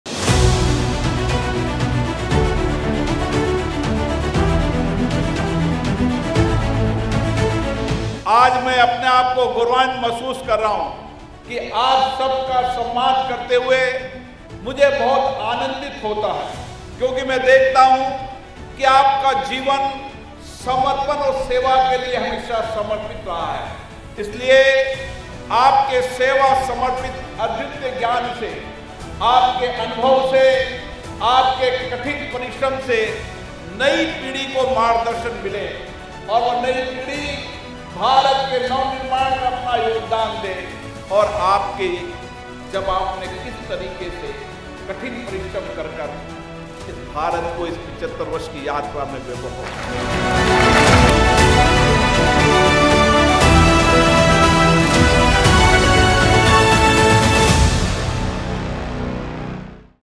शिक्षक एवं सेवानिवृत्त गौरव समारोह - एक झलक
संसदीय क्षेत्र कोटा में शिक्षक एवं सेवानिवृत्त विभूतियों को सम्मानित किया। सेवानिवृत्त विभूतियों ने देश-प्रदेश की सेवा में अपना जीवन समर्पित किया। कठिन परिश्रम और कठोर मेहनत कर नए भारत के निर्माण में अपना बहुमूल्य योगदान दिया, ऐसे सभी कर्मयोगियों को हम नमन करते हैं। शिक्षक जिन्दगी भर के तप व समर्पण से ऐसे क्षमतावान और संस्कारवान विद्यार्थी तैयार करते हैं जो देश के नवनिर्माण का बीड़ा उठाते हैं। वे विद्यार्थियों में सपने देखने, उनको पूरा करने का सामर्थ्य उत्पन्न करते हैं। विकसित भारत के संकल्प की सिद्धी में उनकी भी अहम भूमिका होगी। सेवानिवृत्त अधिकारियों-कर्मचारियों और शिक्षकों का आह्वान किया कि वे अपने अनुभव और ज्ञान से नई पीढ़ी को लाभान्वित करें। युवा पीढ़ी का मार्गदर्शन करें। उनमें वह आत्मविश्वास उत्पन्न करें जिससे वे नए प्रण और नए संकल्प के साथ 2047 तक विकसित भारत के निर्माण के संकल्प की सिद्धी करें।